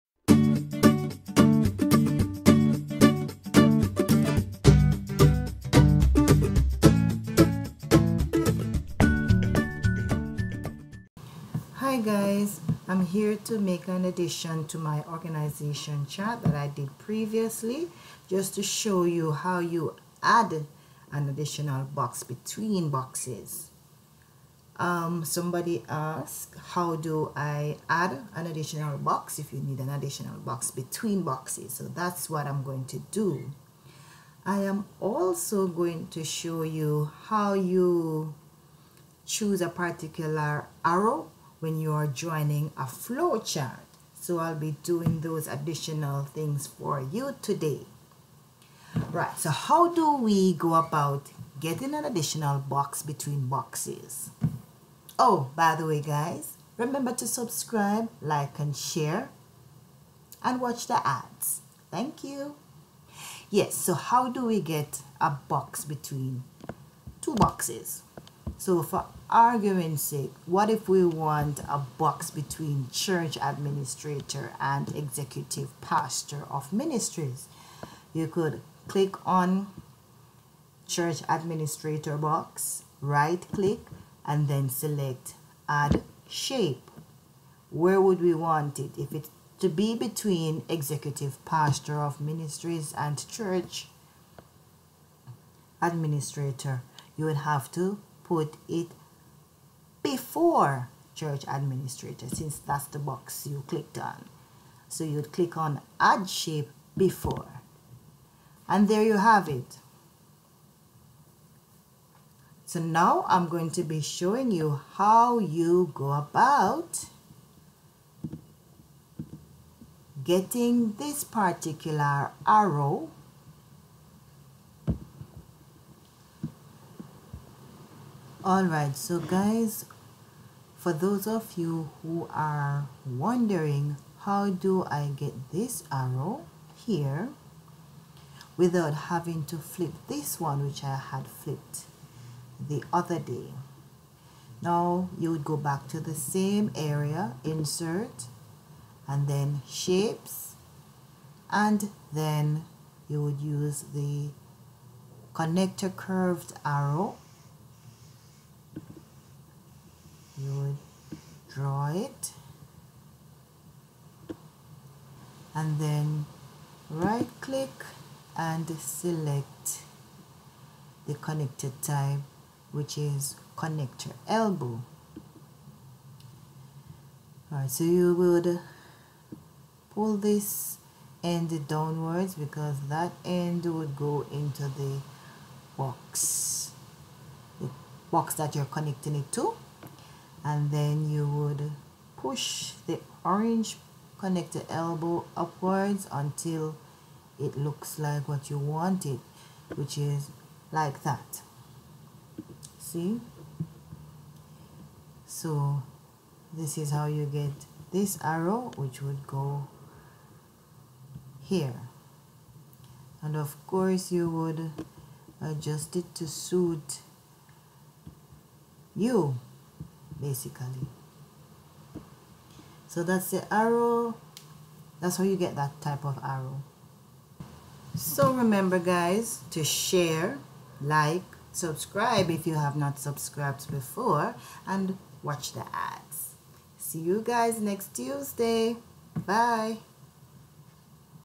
How to create organization and flow chart continued
This video shows additional information regarding creating organization and flow chart in Microsoft Word.